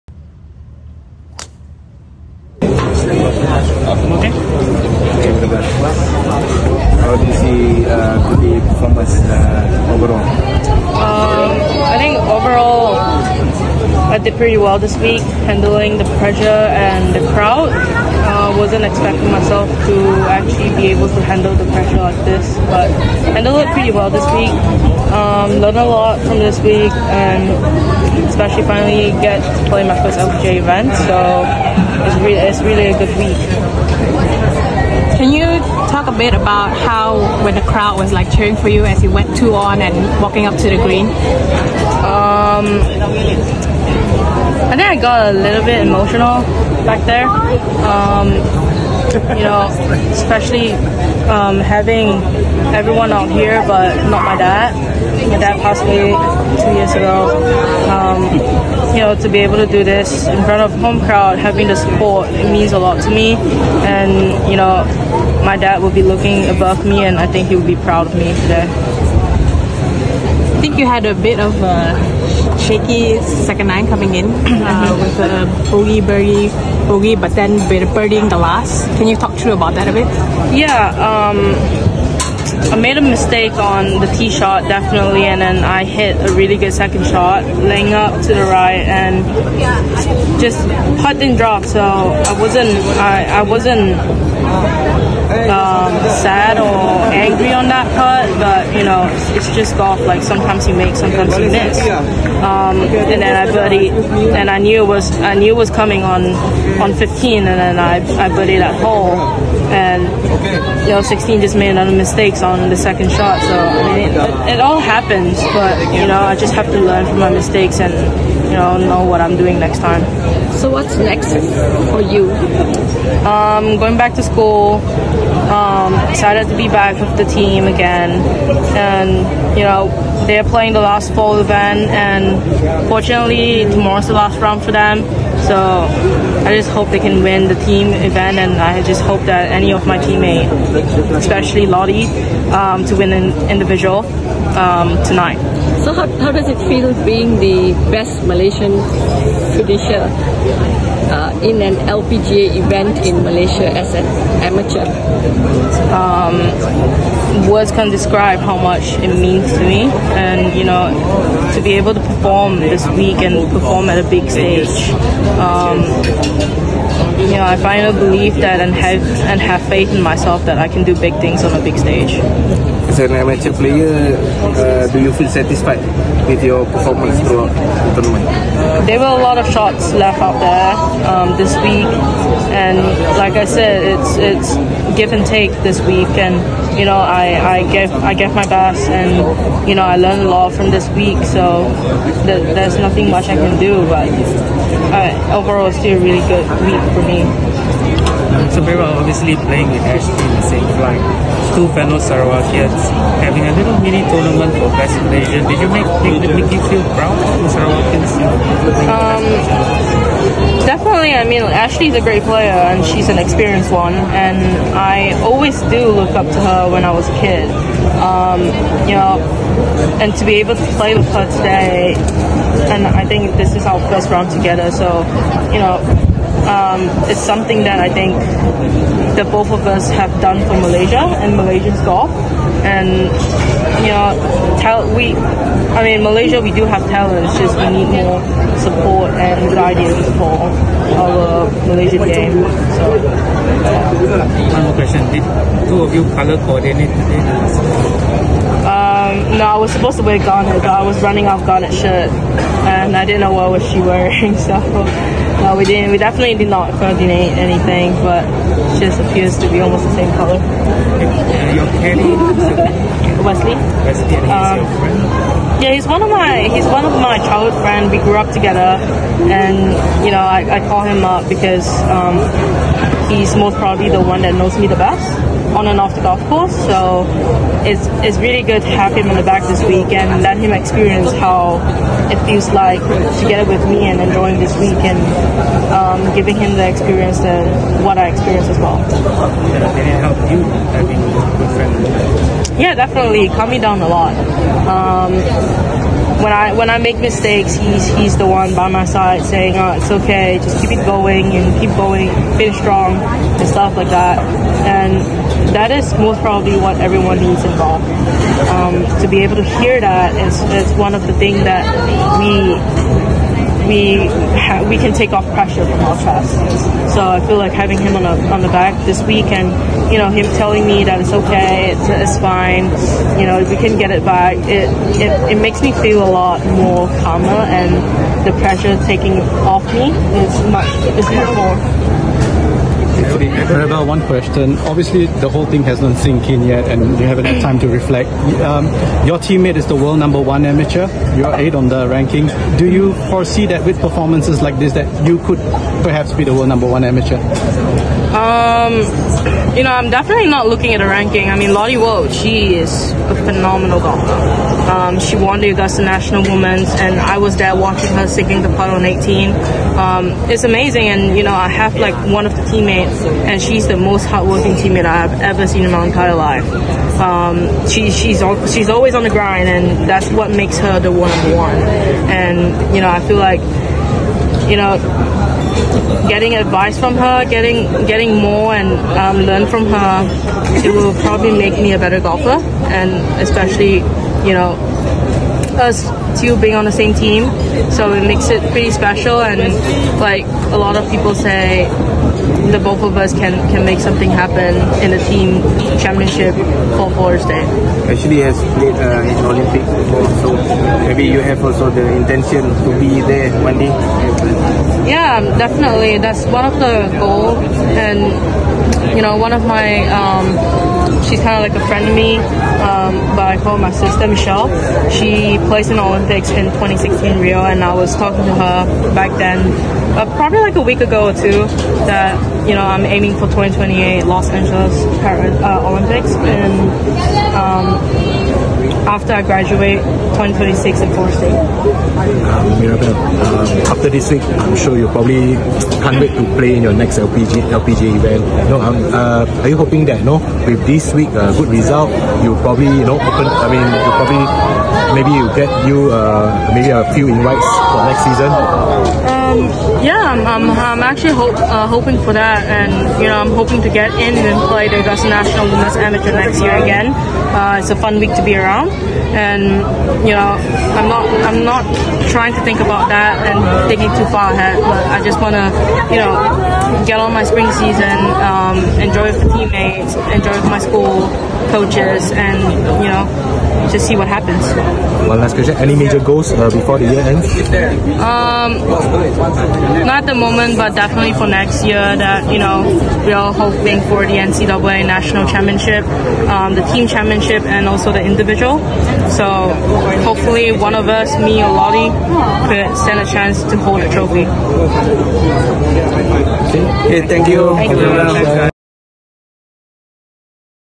Malaysian amateur Mirabel Ting The best Malaysian finisher in an LPGA Tour event in Malaysia.
Malaysian amateur Mirabel Ting carded a final round 2-under 70 for total 12-under 276 for joint 12th position. The best Malaysian finisher in an LPGA Tour event in Malaysia.